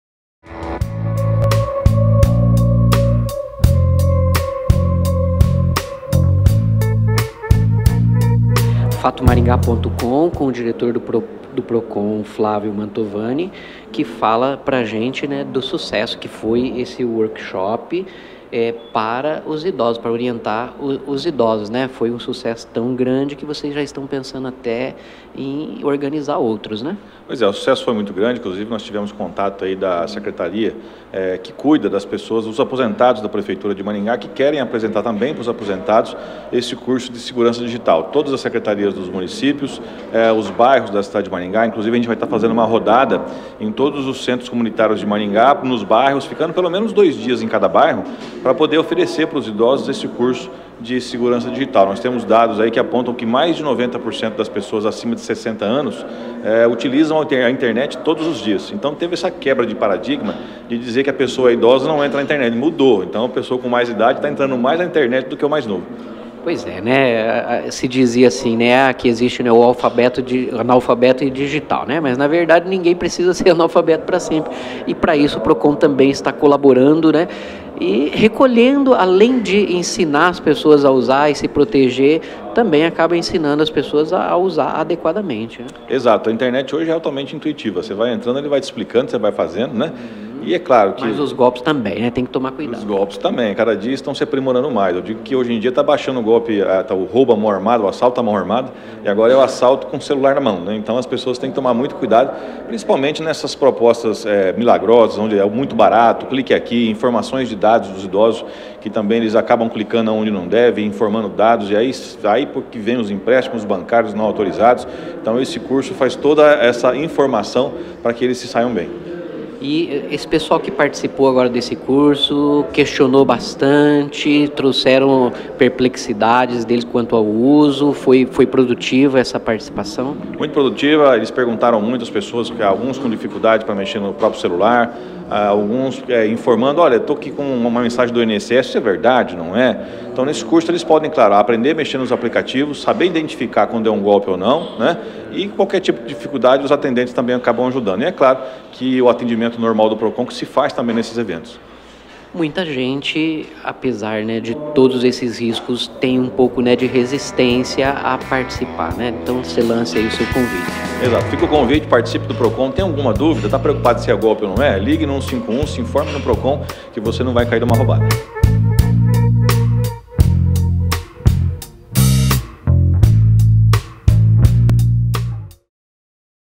Procon vai realizar novos cursos sobre segurança na internet para idosos
"Mais de 90% das pessoas acima de 60 anos usam a interne todos os dias, por isso vamos levar curso a todos os centros comunitários de Maringá", diz o coordenador Flávio Mantovani

O sucesso de participação do workshop de orientação de proteção digital para idosos realizado pelo Procon de Maringá, já está fazendo a instituição que defende os direitos do consumidor a organizar novas edições.

Na primeira edição realizada na metade do mês de agosto, o curso reunião 30 idosos que puderam aprender  muitas coisas do mundo digital, como o funcionamento de aplicativos e como identificar mensagens nocivas e enganosas.

O diretor Flávio Mantovani ainda não confirma a nova data, mas disse em entrevita a OFATOMARINGA.COM que o interesse foi tão grande que o curso poderá se repetir várias vezes e quantas vezes forem necessárias.

"O sucesso foi muito grande e atraiu a atenção até do próprio serviço público. Vamos organizar uma edição que vai atender os aposentados da prefeitura. Além disso, vamos levar o curso em todos os centros comunitários de Maringá. Temos dados que indicam que mais de 90% das pessoas acima de 60 anos utilizam a internet todos os dias quebrando o paradigma que dizia que a pessoa idosa não usa internet", explica Mantovani.

É justamente o aumento do interesse dos idosos pela internet que fez com que o número de tentativas e modalidades de golpes aumentasse, e segundo o coordenador do Procon, o workshop sobre segurança digital é uma arma preciosa para evitar que as pessoas sejam levadas a enganos.

O Procon maringaense tem agenda de eventos e orientações para diferentes segmentos da sociedade, seja consumidores ou empresas. O objetivo é que o Código de Defesa do Consumidor (CDC) seja respeitado nas relações de consumo.